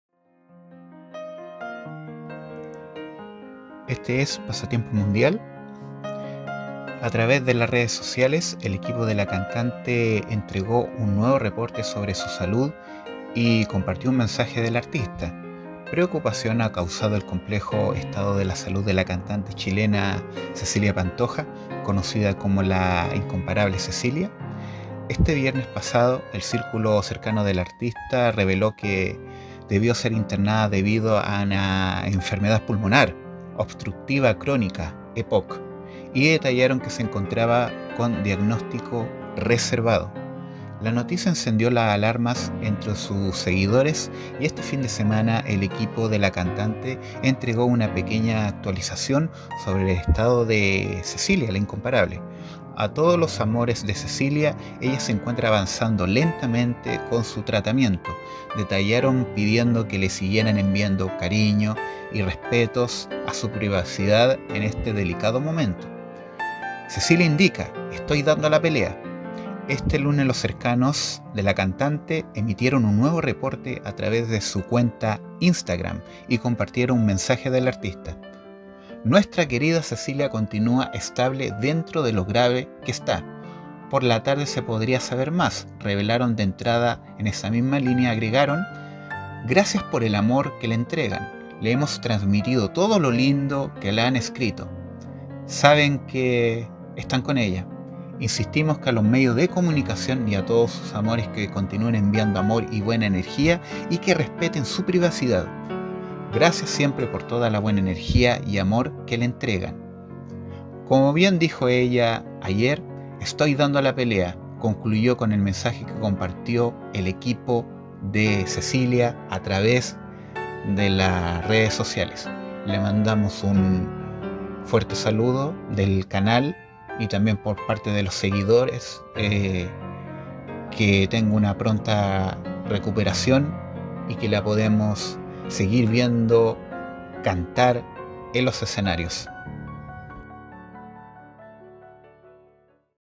CECILIA LA INCOMPARABLE DANDO UNA DURA PELEA ESTADO DE SALUD
LA NUEVA OLA CHILENA CON CECILIA LA INCOMPARABLE POR SU ESTADO DE SALUD.
DIFICILES MOMENTOS DE LA CANTANTE CHILENA DANDO UNA DURA PELEA.

EDICION Y NARRACIÓN DE VIDEO ROBERTO BUSTOS SCHIFFERLI.
COMPARTE Y PRESIONA LA CAMPANITA.
SE AGRADECE SI PUEDES REALIZAR UNA DONACIÓN EN EL BOTON GRACIAS DEL VIDEO